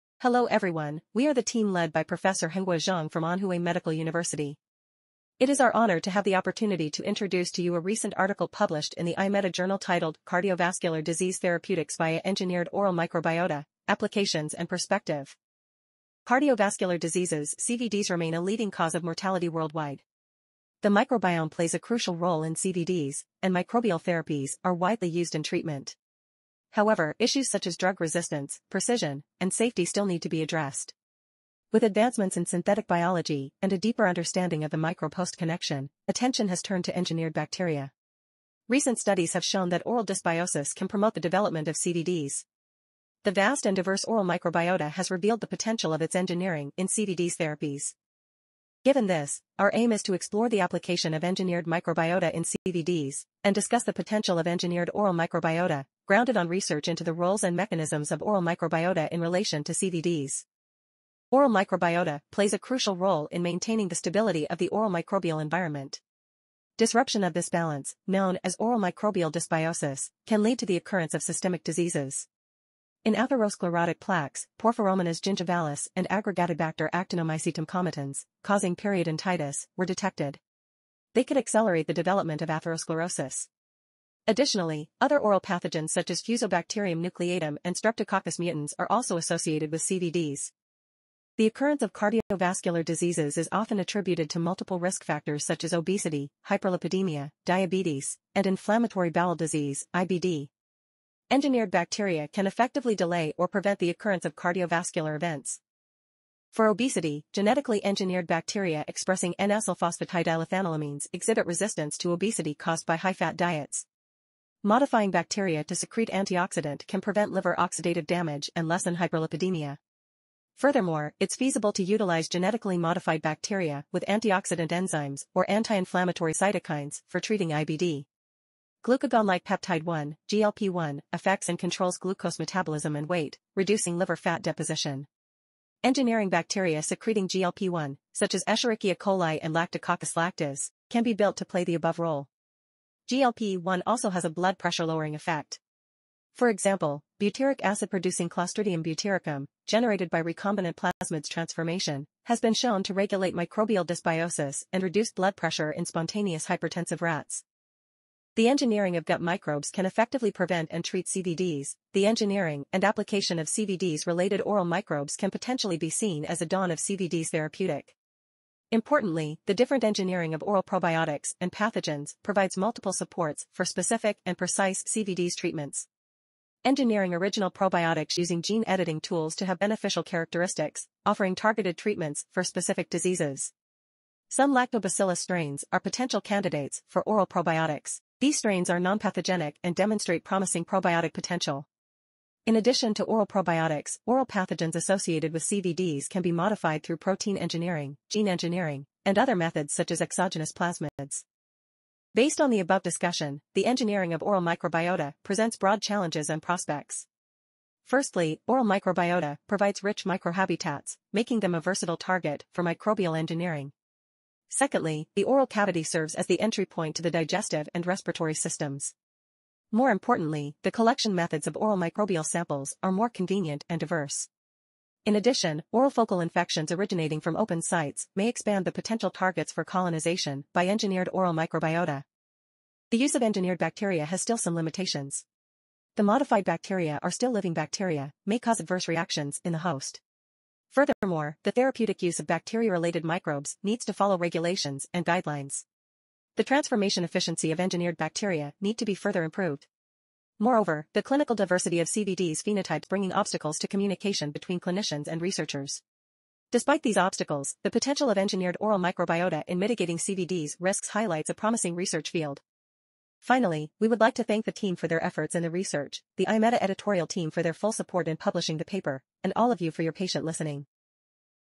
iMeta | Cardiovascular disease therapeutics via engineered oral microbiota
Graphical Abstract
Engineering bacteria are considered as a potential treatment for cardiovascular diseases and related risk factors. Oral bacteria are closely related to the occurrence and development of cardiovascular diseases, and their engineering has broad prospects and potential in the treatment of cardiovascular diseases. Oral pathogenic bacteria undergo protein and genetic engineering, including the incorporation of exogenous plasmids to yield therapeutic effects; genetically engineered oral probiotics can be harnessed to secrete cytokines and reactive oxygen species, offering novel therapeutic avenues for cardiovascular diseases.